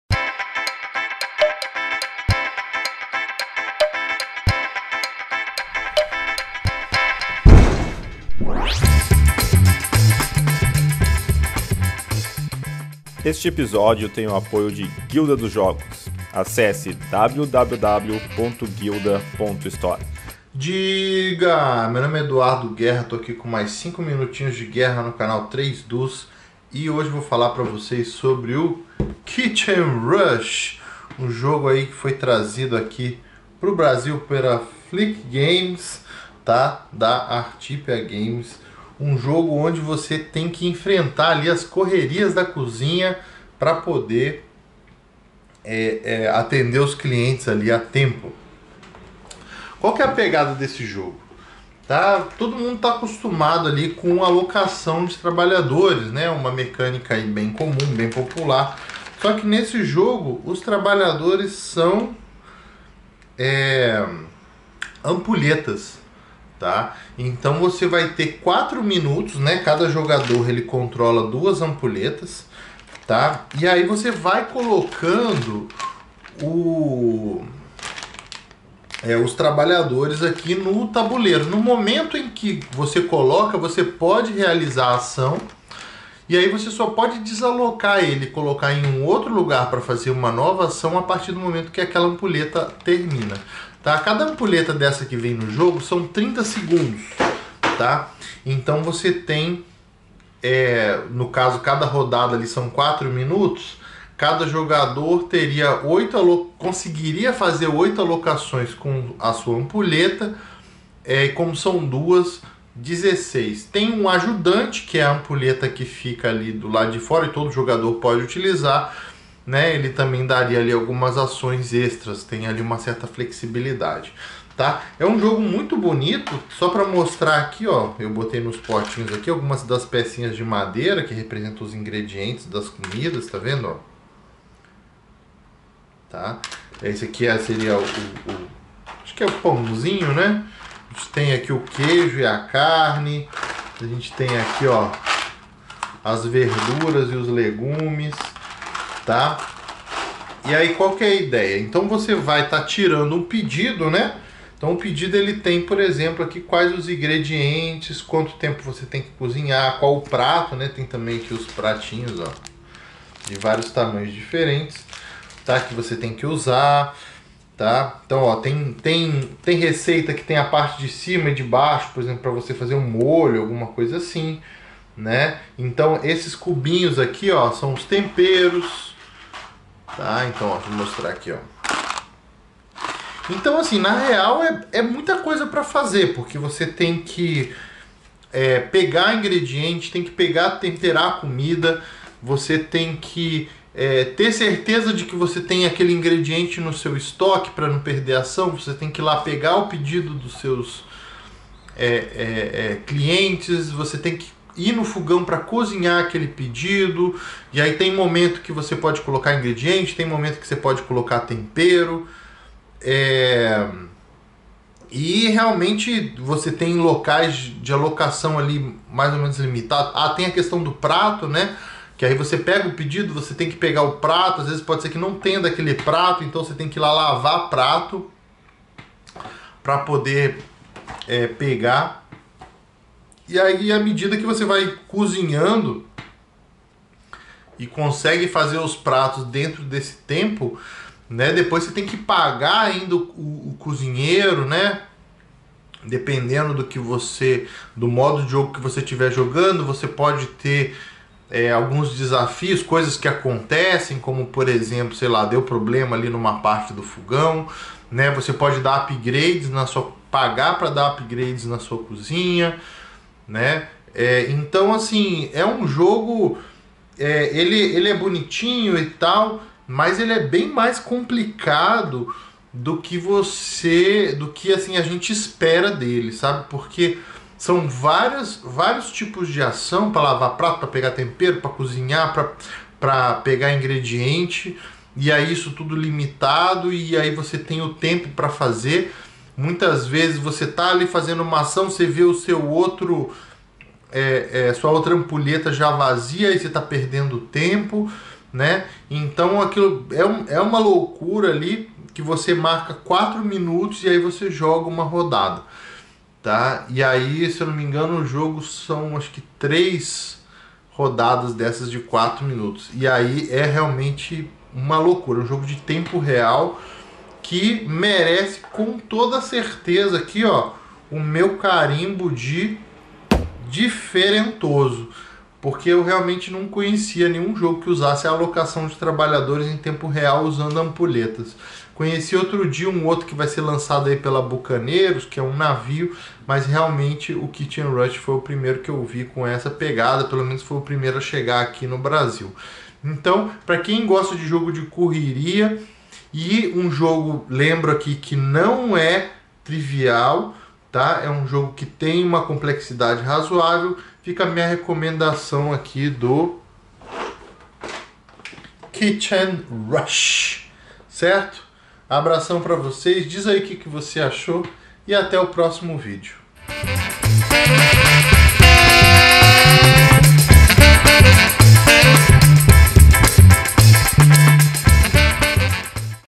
5 Minutos de Guerra - Ep. 86 - Kitchen Rush
Neste episódio Edu Guerra apresenta o Kitchen Rush. Um jogo em tempo real, confere aí.